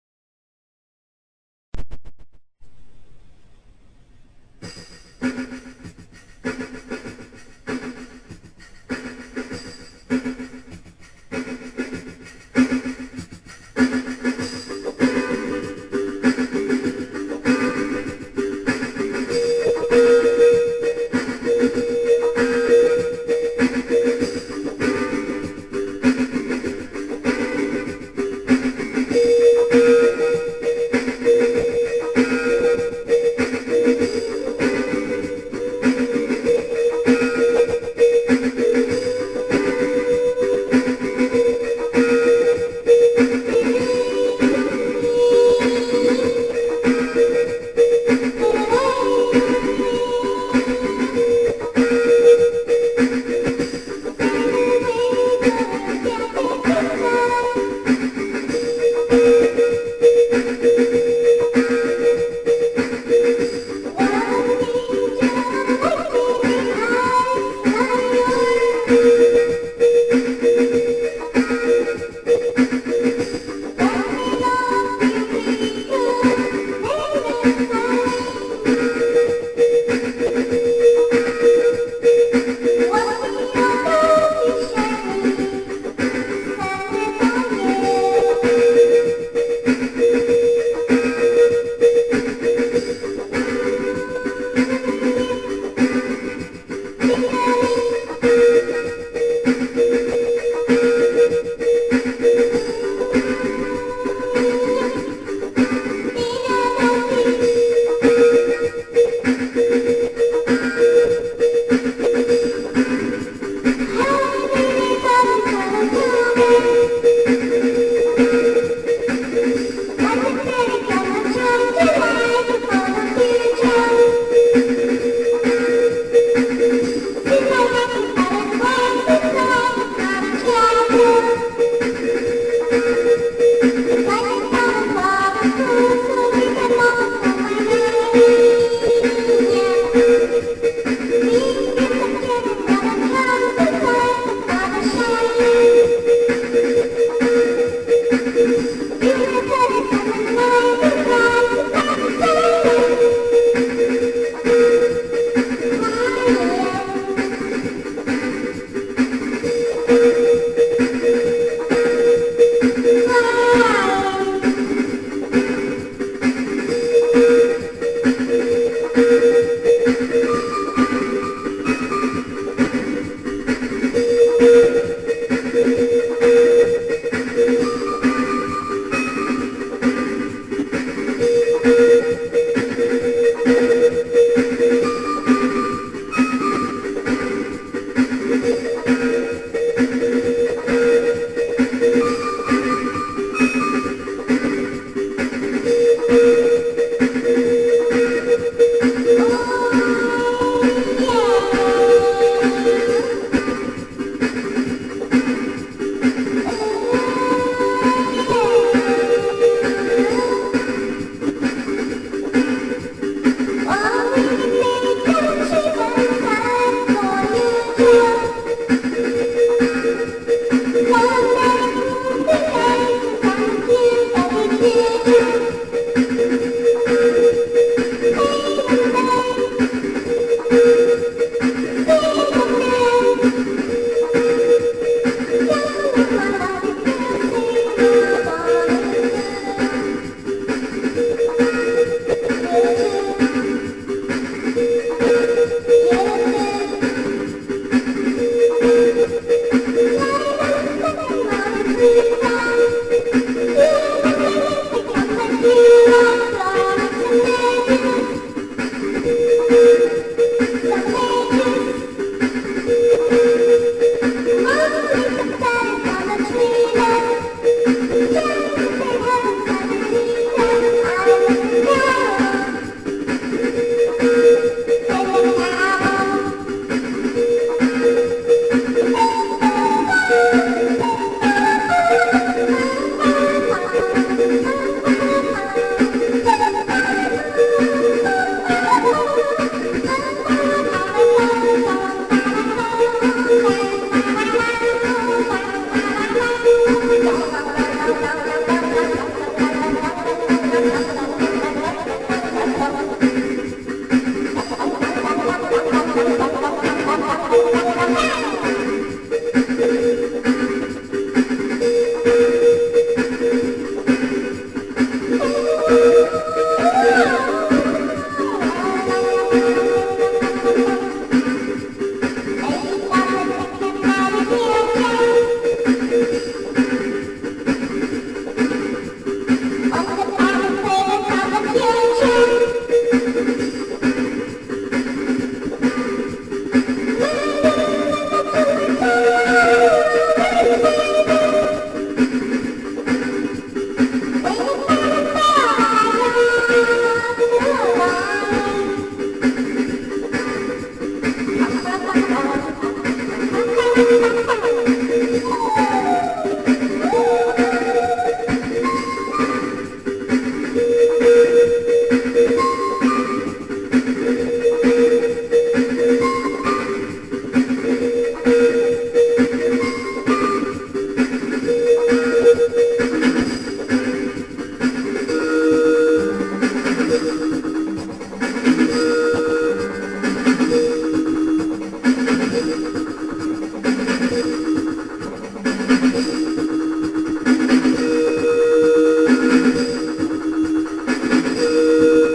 Cats crazy song.wmv
Made from Crazy Talk 6

This cat is  singing my music.
This is combination of original song with programs specials.

Listen and comment.
Nice fun.